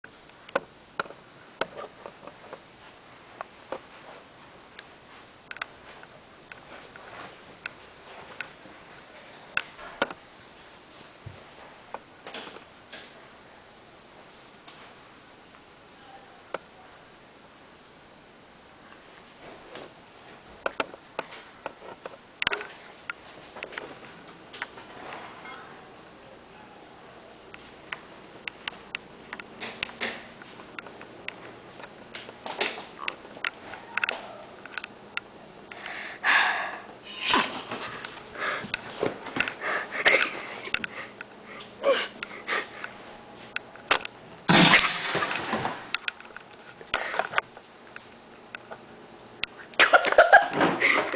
Jeffrey Chen's Massacre
After Jeffrey Chen recieves a B+ on his Midterm final exam for Algebra 2, he goes wild. He breaks into his friend's house where he finds that a study group is taking place. From there...you use your imagination. xDD